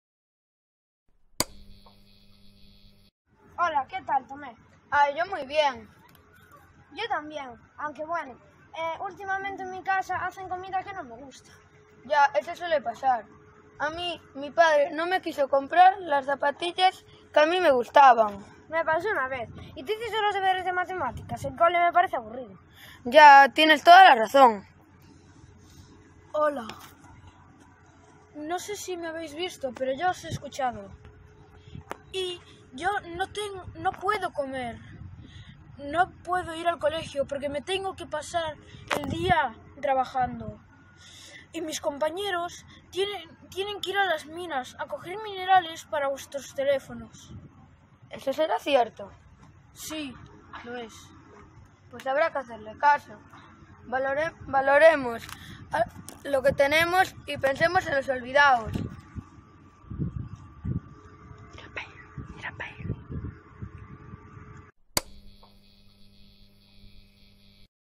Contrastes Injustos
Alumnado de 6º Primaria

Estamos disfrutando de derechos que muchas veces no valoramos. Para otros seres humanos conseguirlos sería su mayor sueño

CEIP Ponte Sampaio 5º e 6º de Primaria
Festival de Clipmetrajes de Manos Unidas